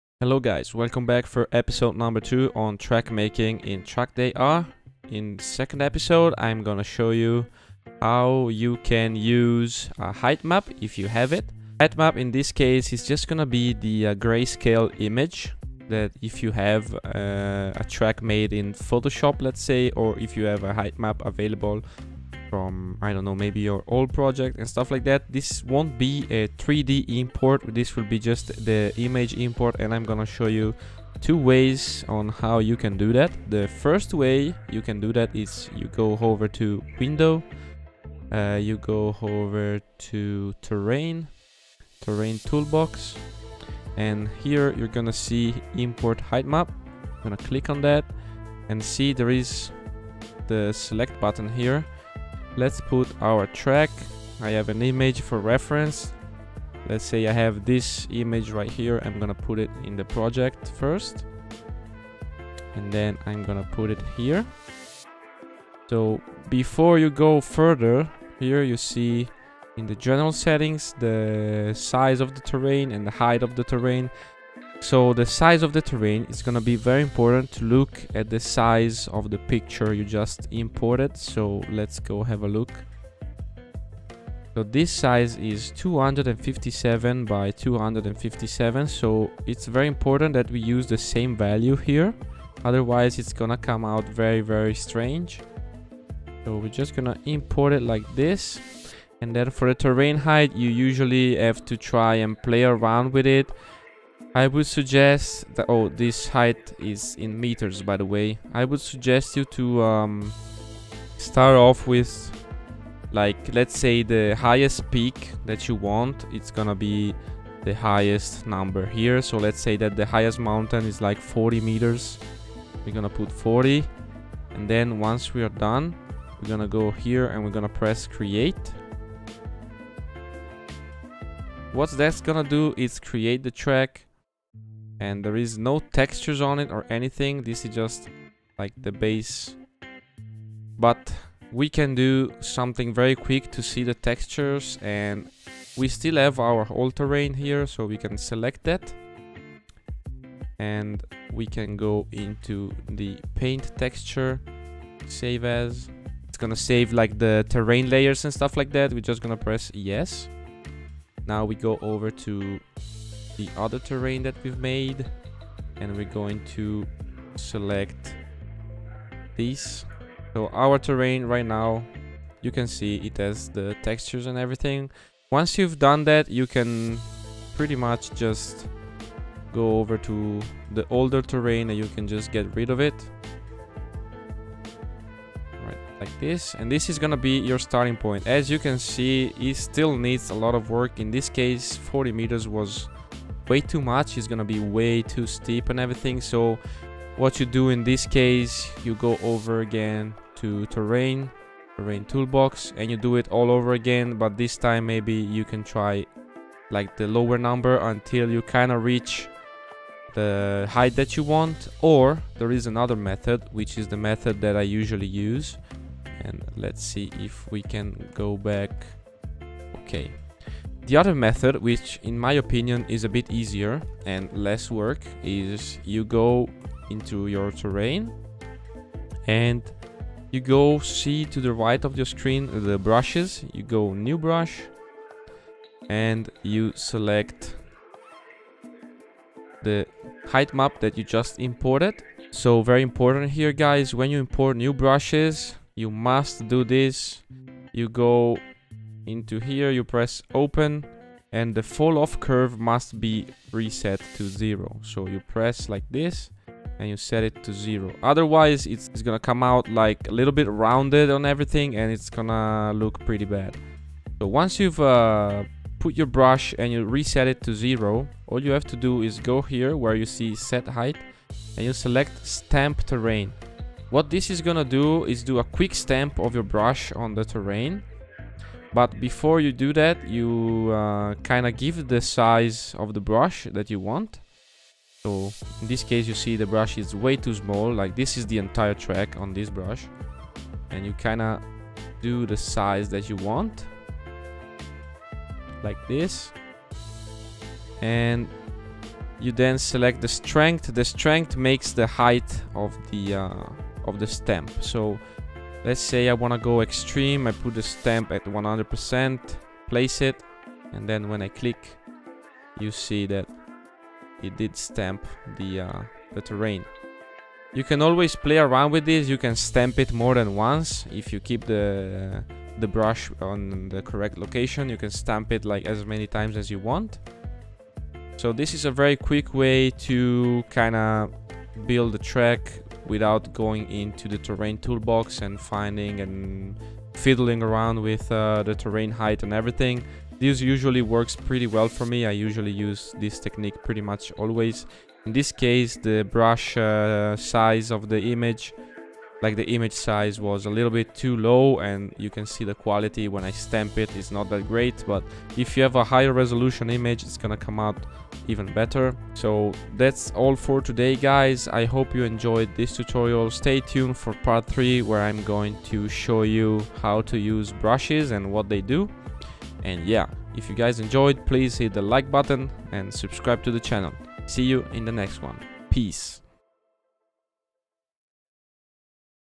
[Tutorial]How to build a track in TrackdayR EP.2
hey there welcome back! in this video  i will show you 2 wayr to import a height map in unity!
hope you enjoy and as always if you need help use the comment section down below!